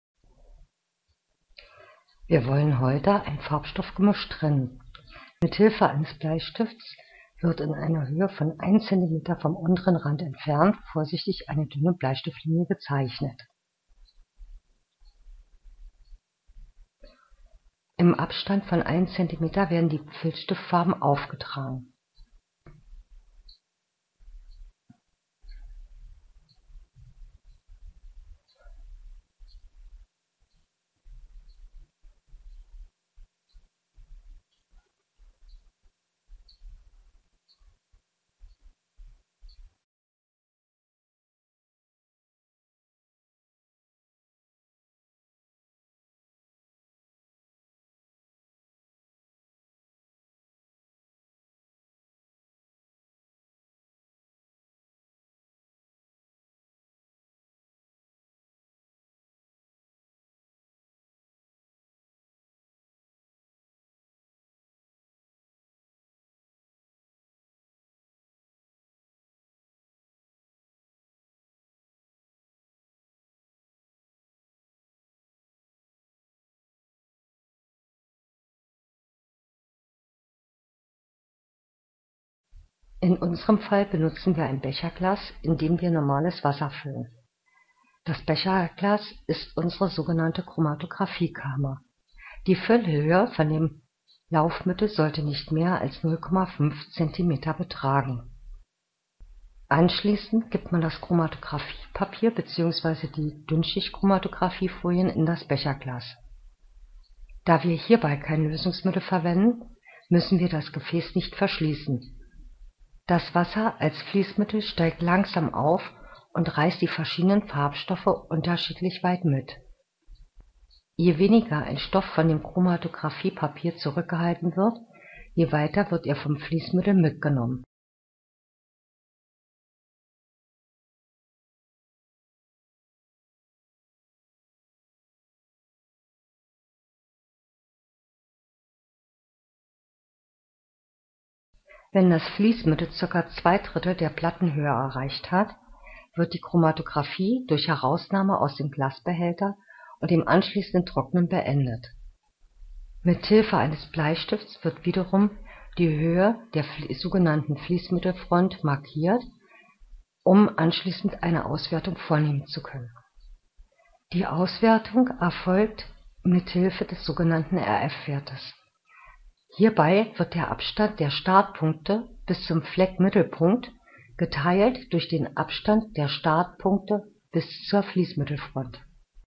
Chemie Chromatographie
Dieses Video zeigt eine typische Stofftrennung mittels Flitration